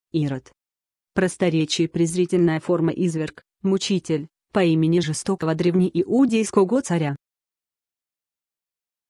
ИРОД - что это такое? значение и описание
Хочешь знать что значит ИРОД? тогда слушай до конца.

 -а, м. (прост, презр.). Изверг, мучитель [по имени жестокого древнеиудейско-го царя].